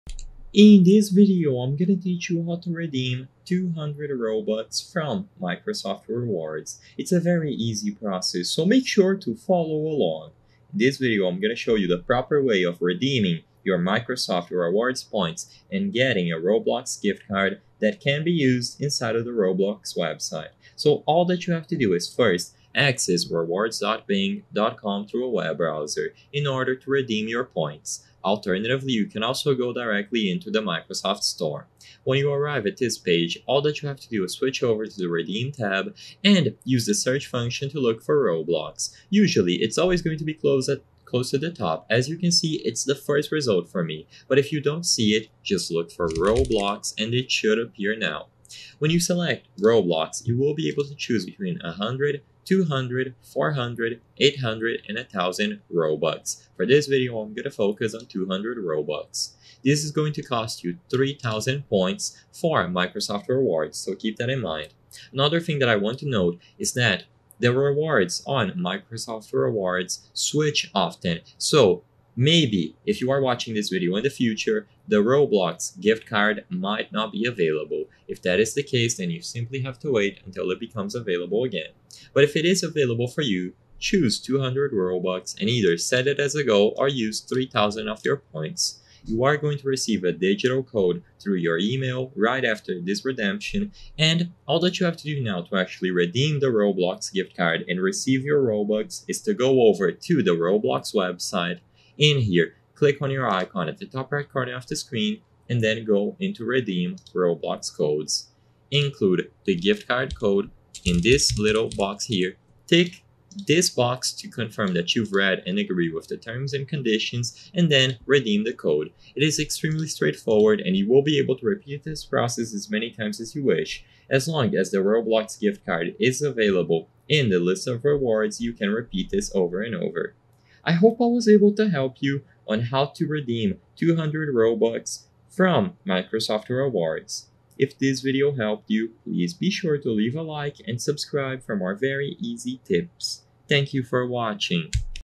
How To Redeem 200 Robux from Microsoft Rewards (2025)
In this video I will clear your doubts about how to redeem 200 robux from microsoft rewards, and whether or not it is possible to do this.